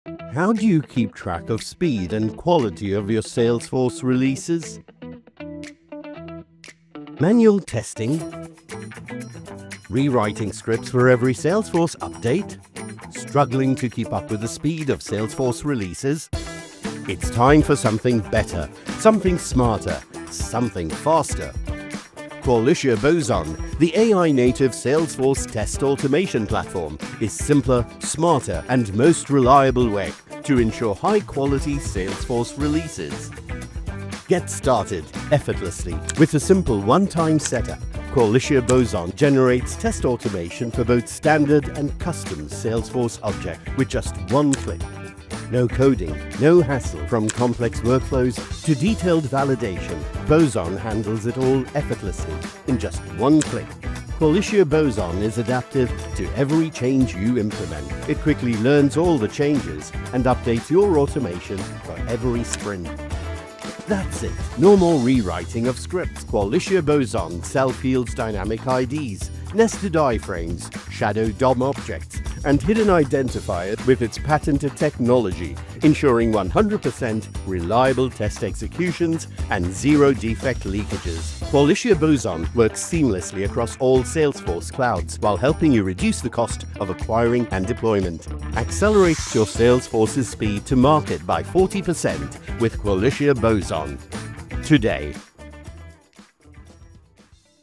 Qualitia Boson - Accelerate Salesforce time-to-market
Qualitia Boson is an AI Native Salesforce test automation platform designed to test Salesforce releases better and faster, reducing the time and cost of deploying Salesforce.